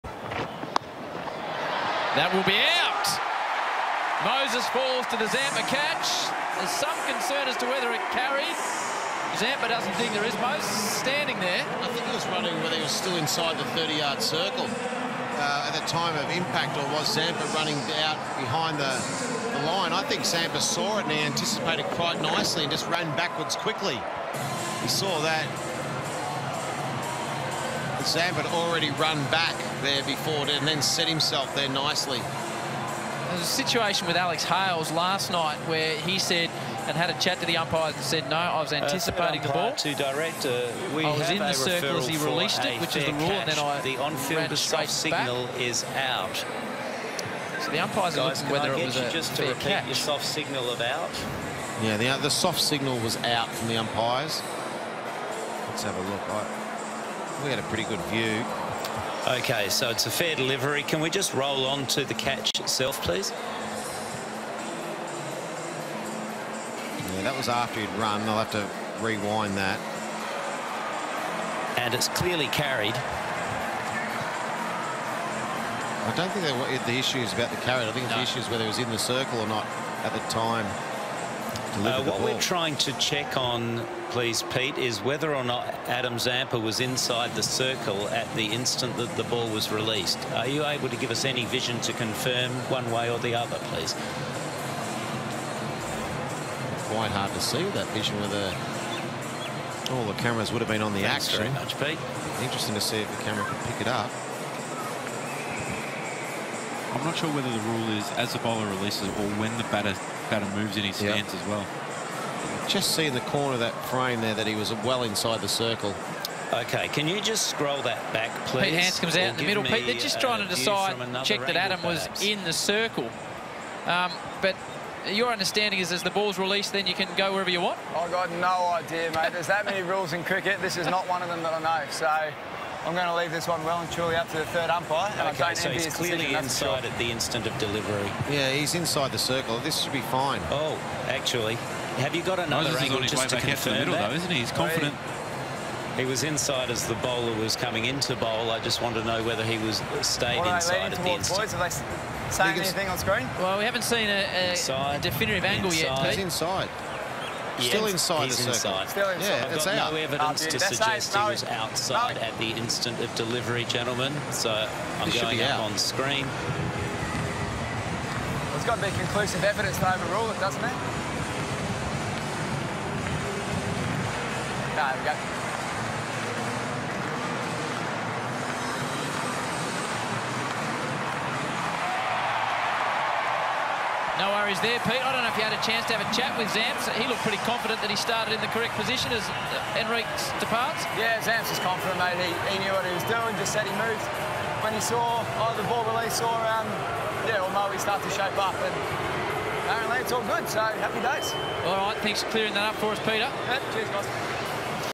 Henriques dismissed after contentious third umpire review
The third umpire needed multiple replays to determine whether or not Moises Henriques should be sent on his way in the BBL Qualifier final at the MCG